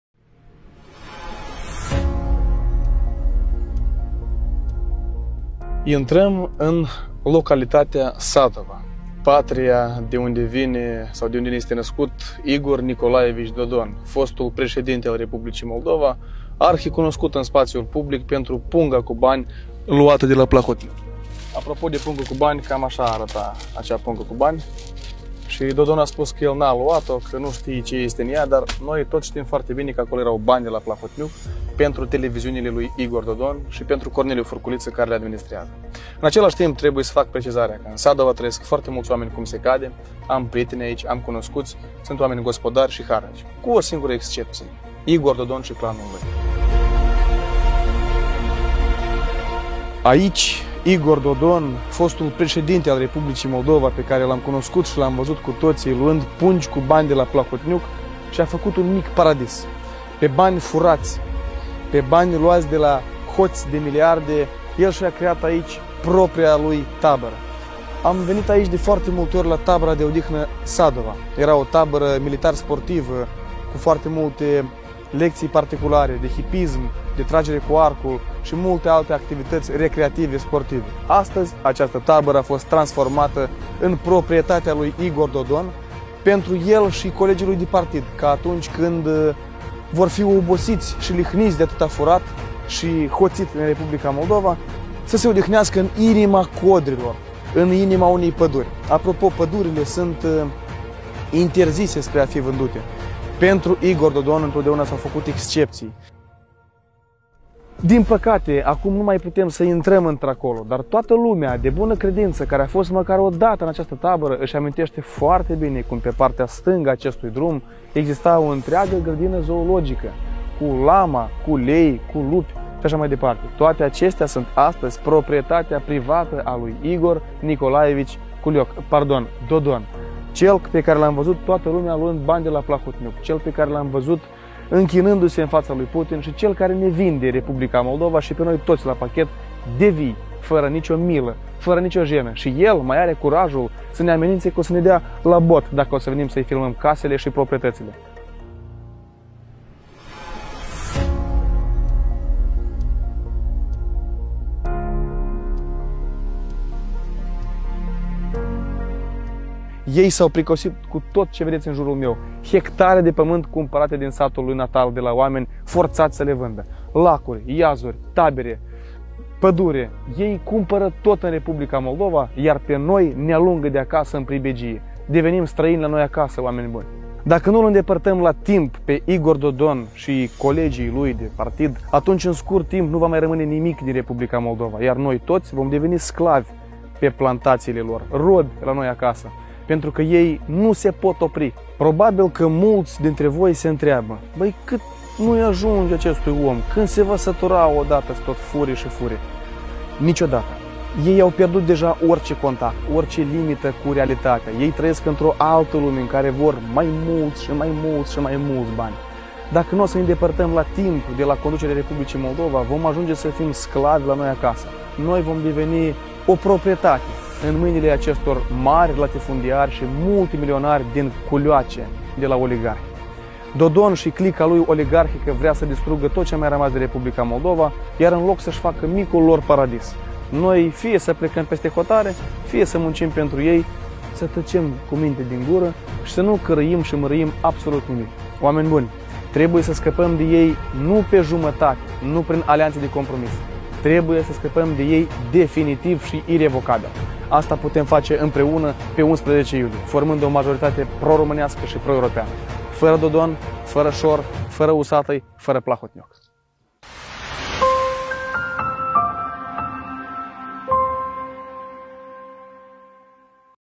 Dodon si Paradisul Imobiliar de la Sadova, Moldova
Din bani furați, Igor Dodon și-a construit un adevărat „paradis” în satul Sadova din raionul Călărași, Moldova. Un exemplu este și tabăra din localitate, care este în prezent la cheremul socialistului și de care dispune în cel mai mârșav mod.
E timpul să punem capăt acestor fărădelegi, pentru că locul lui Dodon este la pușcărie. Pe 11 iulie, alegem România!